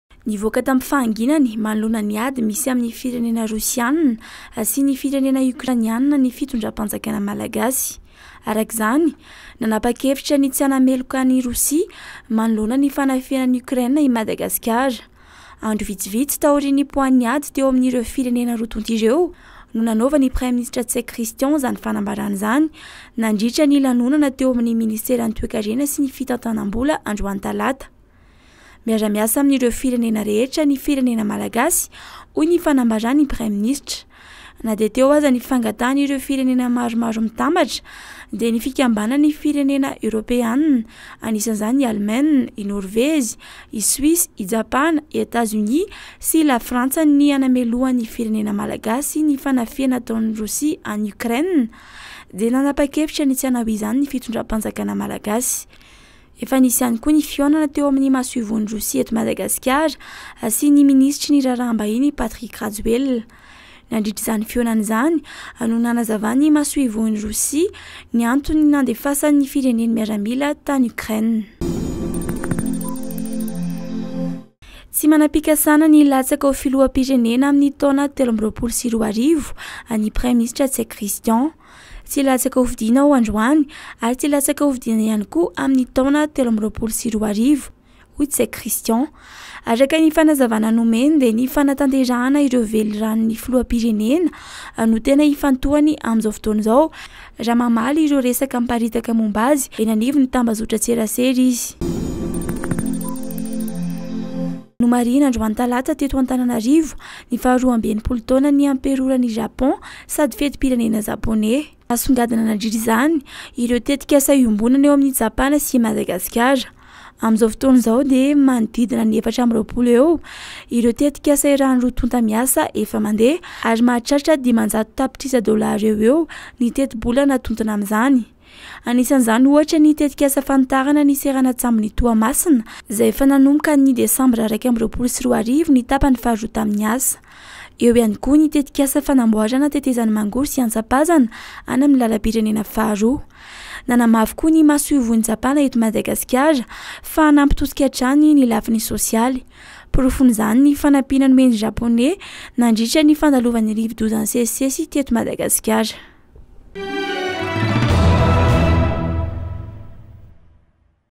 VAOVAO - MINUTE FLASH 01 MARTSA 2022
- ADY : Nanapa-kevitra ny tsy hanameloka an’i Rosia amin’ny fanafihana ataony any Iokrainina i Madagasikara
- FIFIDIANANA : Tsy mikasa ny hilatsaka ho filoham-pirenena i Ntsay Christian
- FIFANDRAISANA AMIN’NY ANY IVELANY : Nasongadina ireo tetikasa iombonan'i Madagasikara sy Japana nandritra ny fankalazana ny faha-62 taonan’ny amperoran’i Japana sady fetim-pirenena japoney